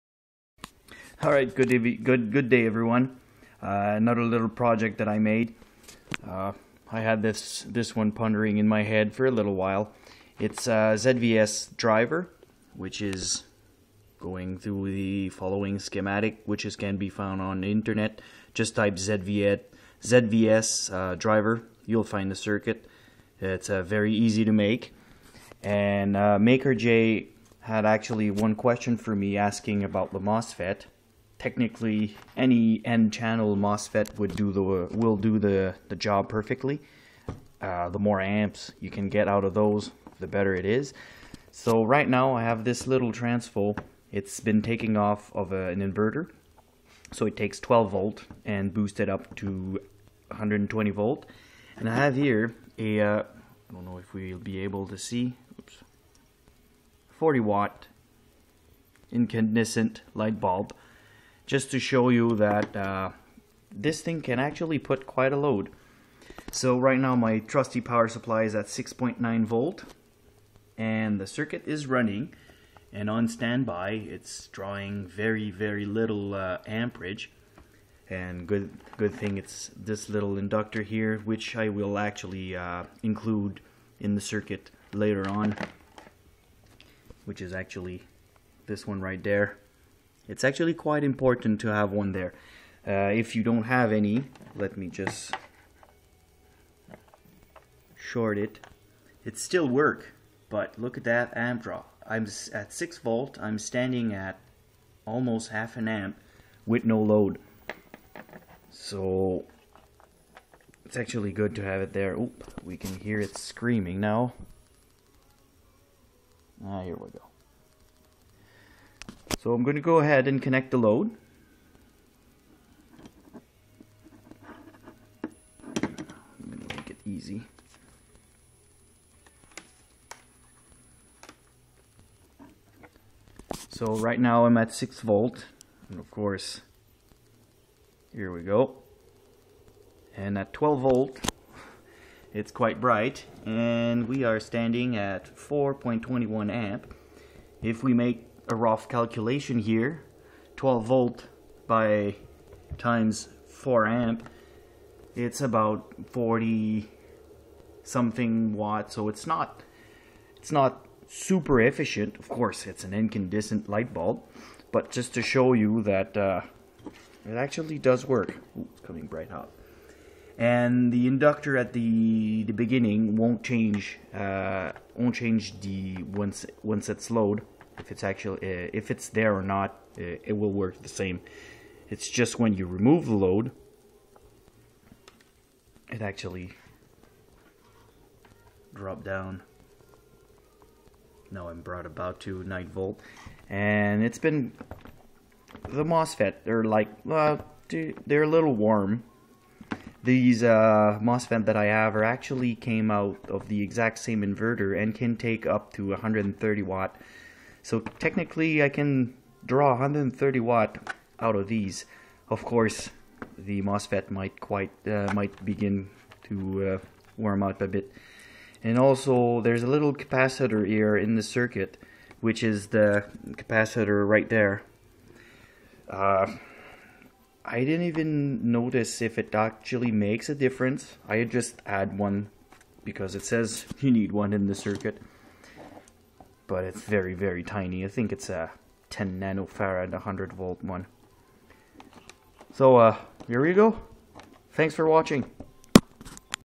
ZVS Driver 12 v to 120 v
Here lates project a small zvs driver that drive a transformer from an old inverter and I show it driving a 40 watts incandescent lamp not the most efficient way to use it but it's simply to show the power capability.